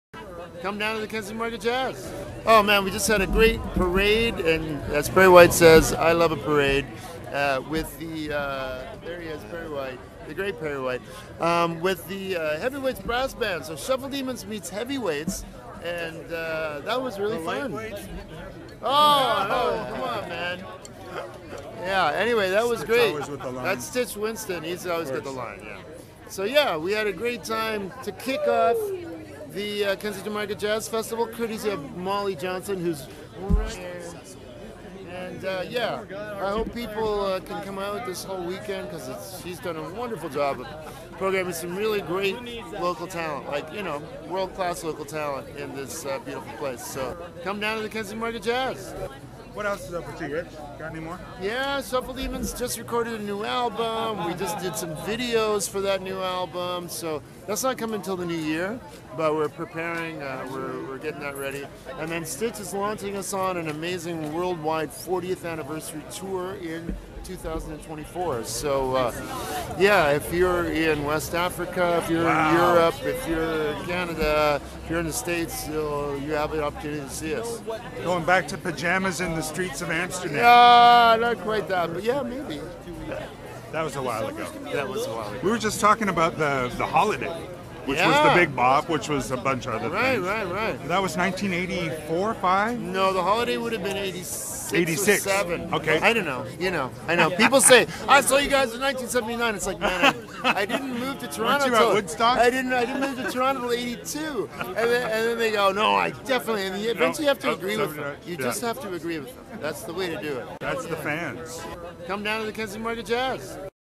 Kensington Market Jazz Festival 2023 with Richard Underhill
at the GardenCar on Augusta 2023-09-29

support local talent!